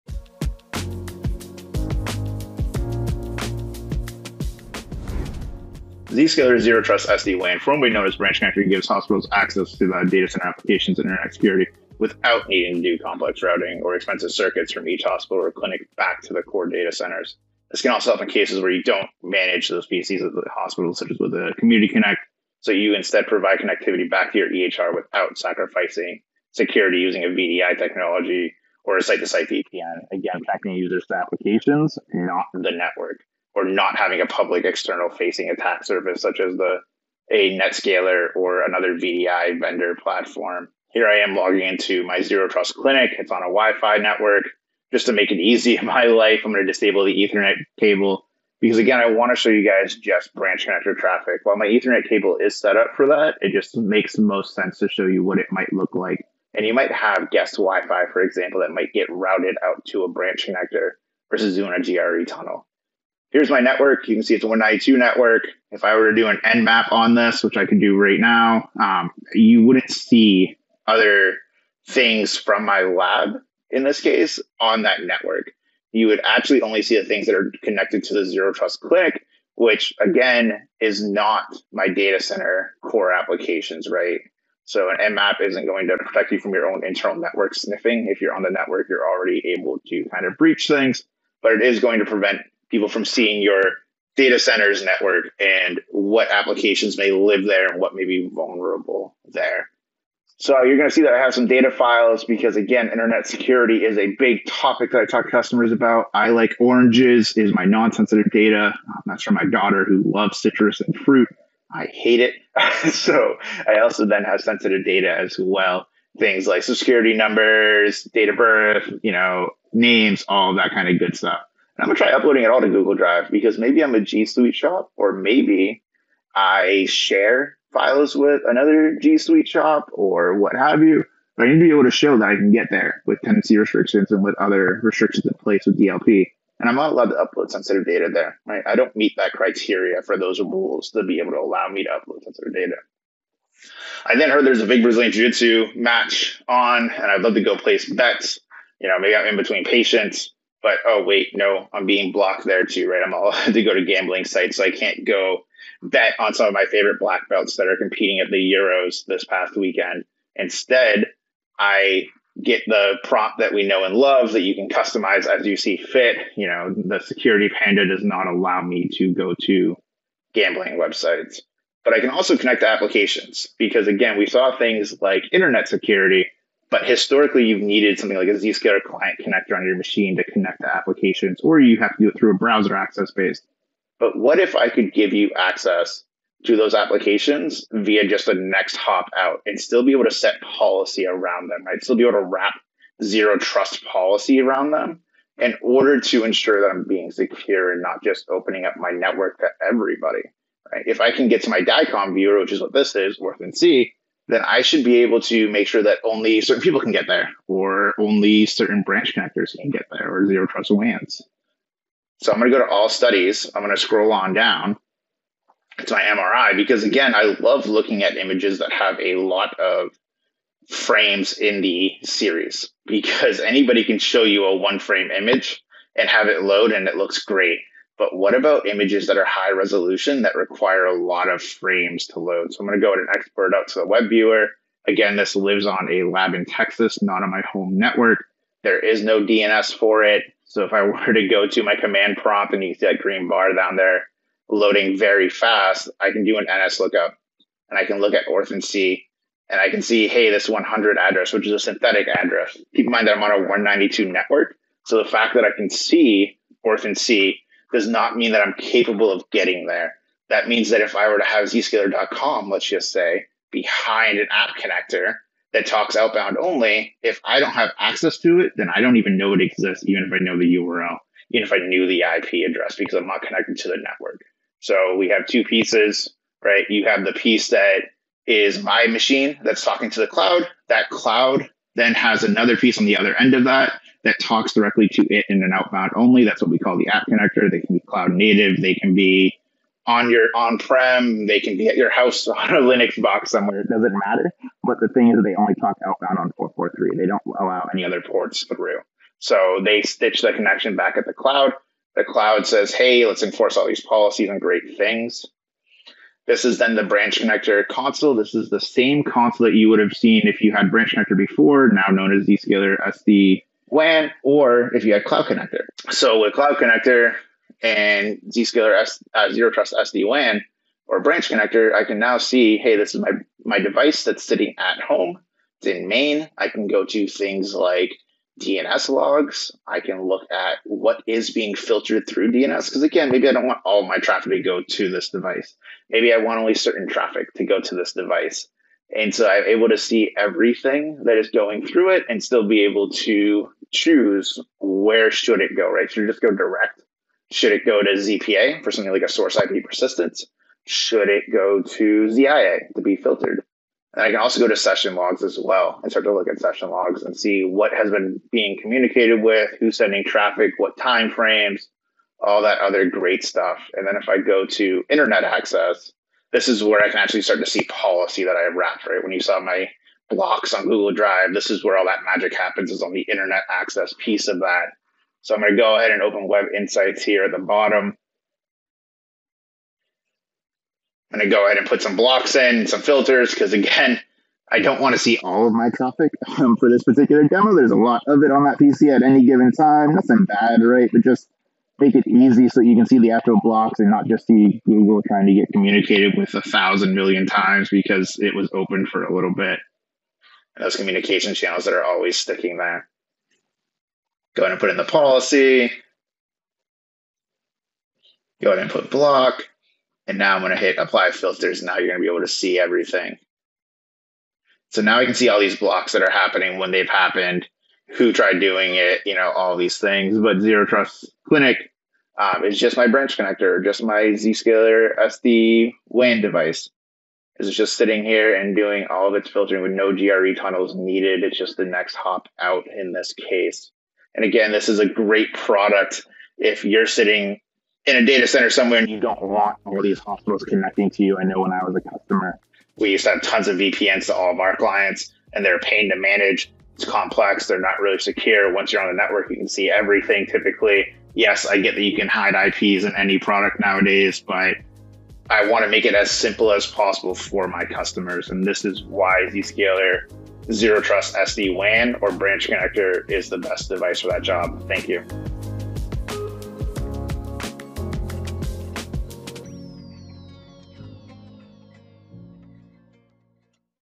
Branch Connector for Healthcare | Zscaler
Enabled by the Zscaler Zero Trust Exchange (ZTE), Zscaler Branch Connector is a virtual machine (VM) that simplifies traffic forwarding to Zscaler services. The Zero Trust Software-Defined Wide Area Network (SD-WAN) is deployed as a Branch Connector VM. This supplies branches and data centers with fast and reliable access to the internet and private applications with a direct-to-cloud architecture. In healthcare, this gives hospitals access to data center applications and their security without needing new complex routing or expensive circuits from each hospital or clinic back to the core data centers.

for more information about how Zscaler enables healthcare organizations to securely modernize and accelerate cloud adoption, improving patient outcomes.